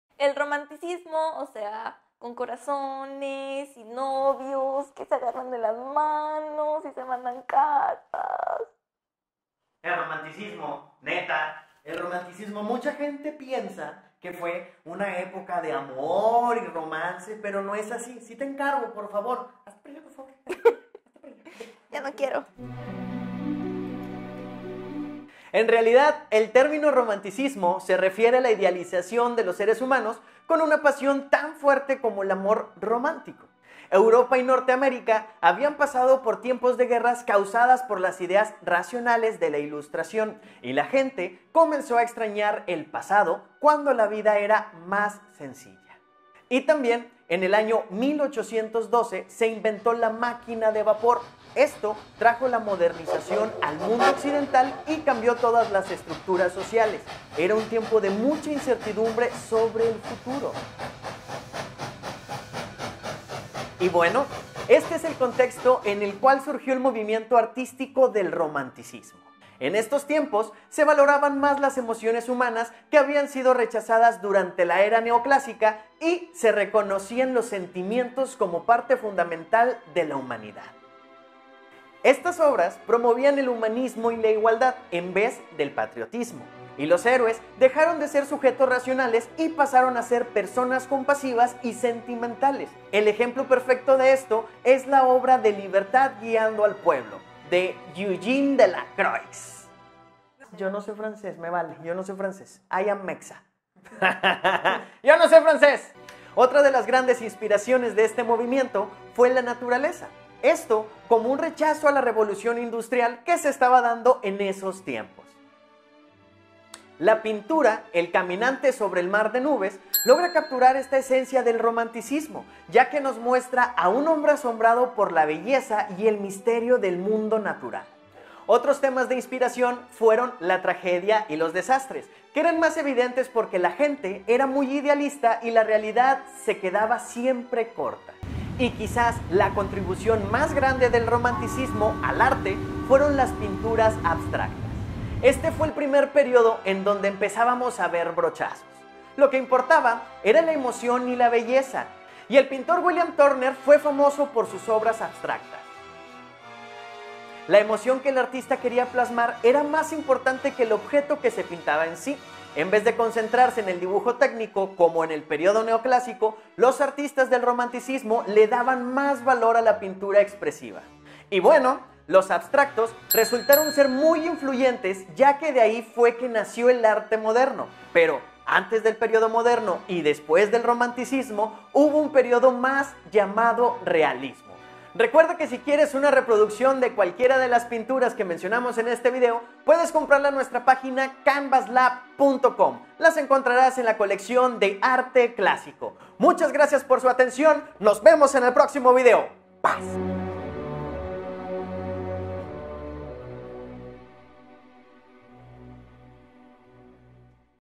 Serie del Arte: "Romanticismo" - Canvas Lab
Muchos creen que el Romanticismo fue una época de mucho amor y romance, pero la cosa no era así 😨 Descubre en este video qué es en realidad el Romanticismo 💔